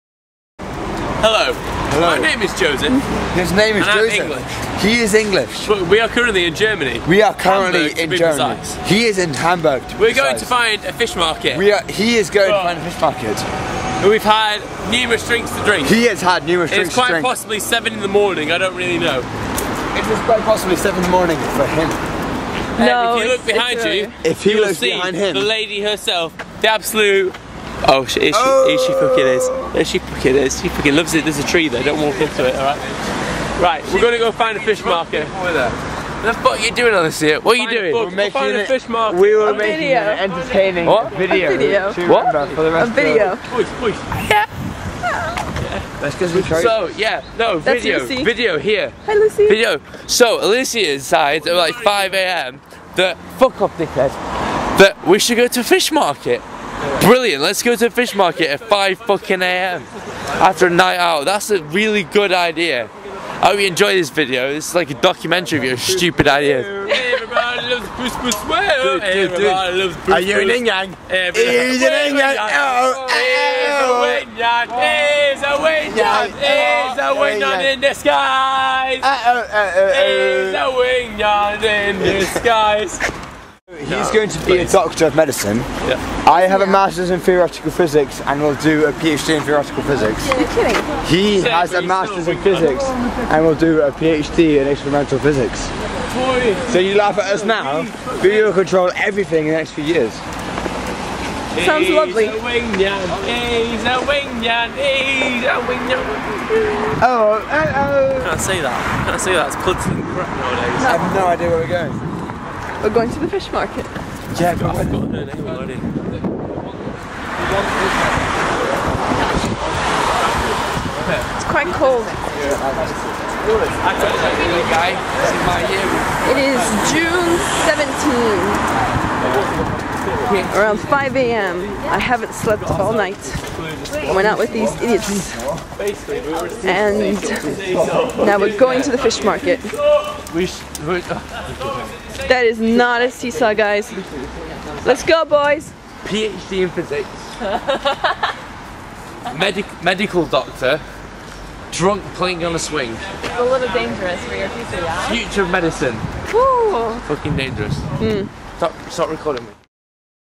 Hamburg Madness
Three Manchester born Englishmen walking around Hamburg at 5 am to find the Fish Market